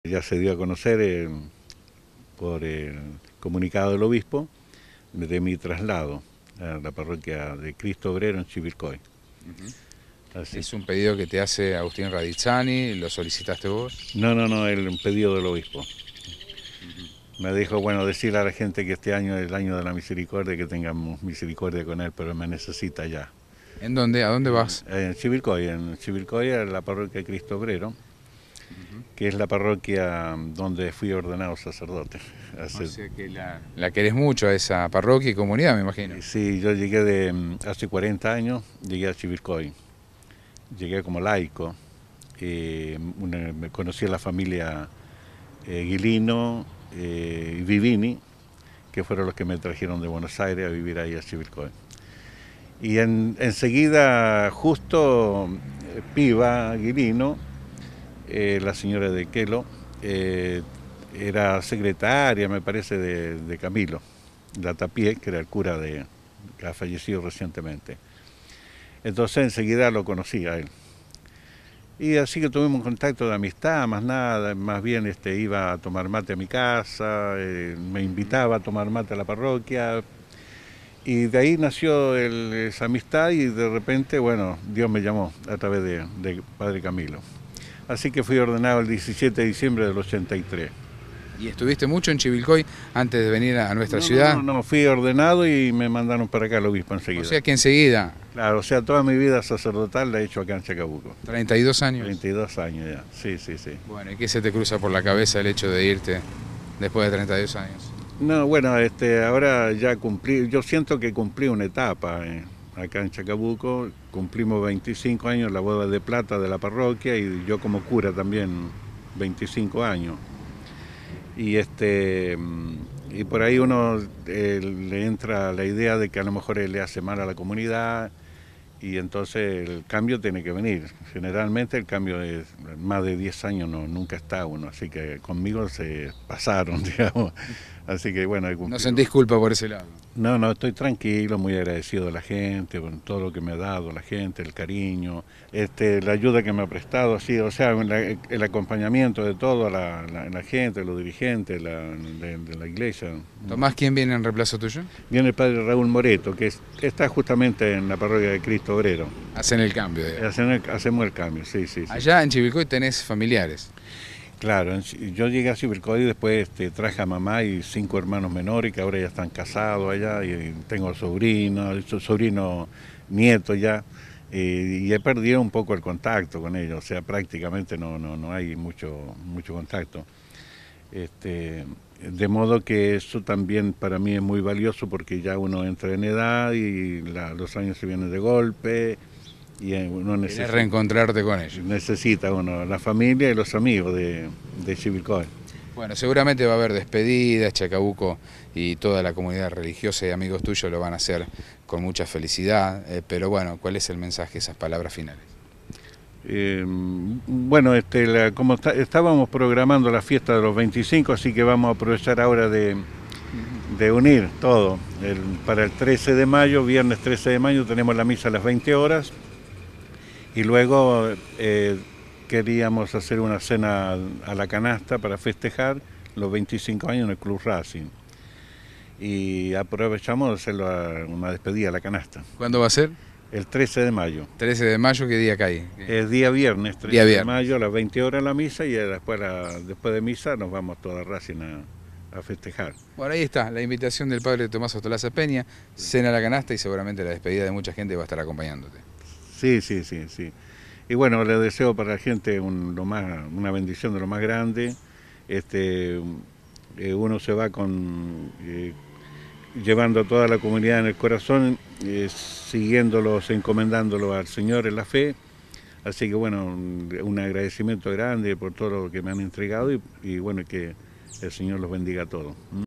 PADRE TOMAS
El párroco que está erradicado desde hace mas de 30 años en nuestra localidad, va a ser trasladado a la ciudad de Chivilcoy - 15 de Abril de 2016